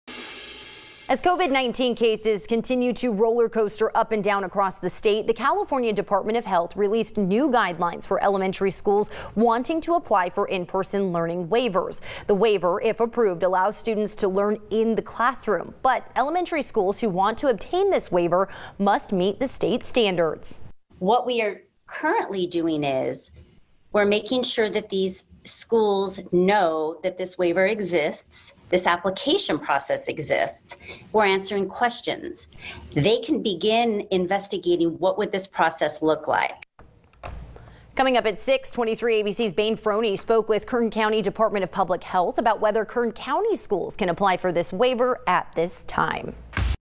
Elementary schools can apply for in-person learning waiver
As COVID-19 cases continue to roller-coaster up and down across the state.  The California Department of Health released new guidelines for elementary schools wanting to apply for in-person learning waivers,